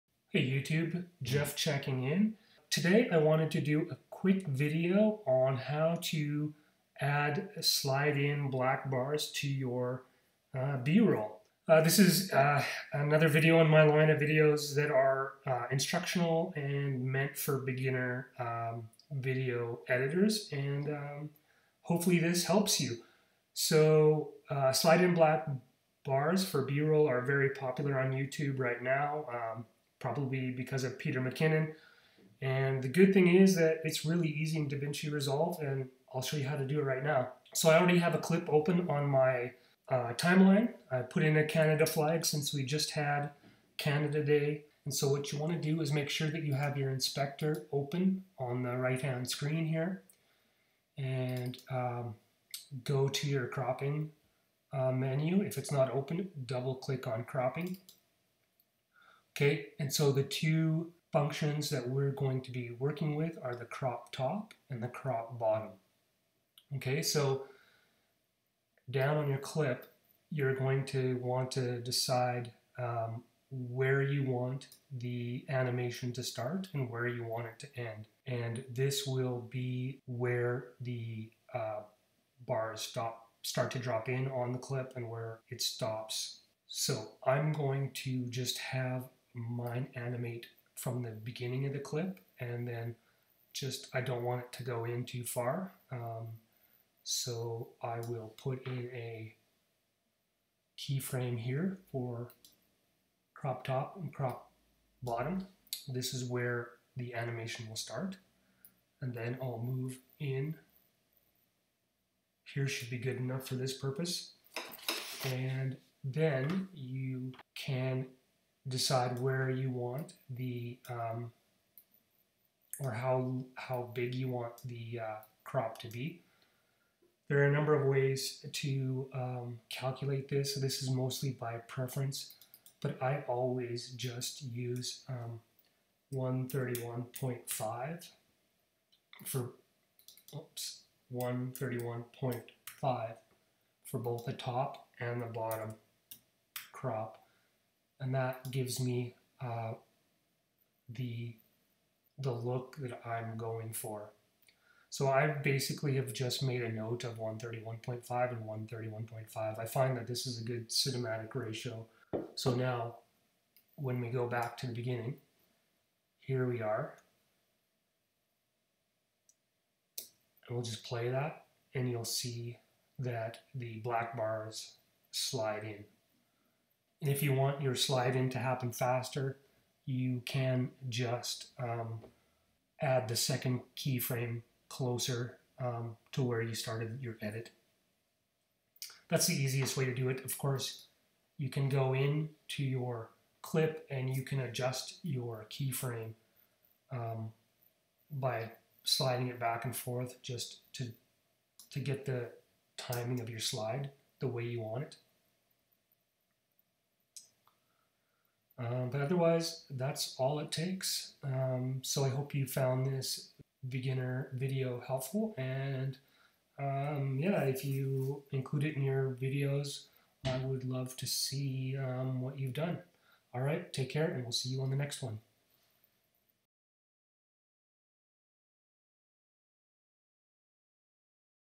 Easy Animate | Slide in Black Bars in DaVinci Resolve 16
In this short tutorial, I show you how to add slide in black bars to your clips to bump up your footage or B-Roll to the next level.

Equipment Used for Vlogs:

Timelapse Camera
Vlog Camera
Timelapse Tripod
Travel Tripod
Travel Camera
L Bracket
Filter System
Digital Voice Recorder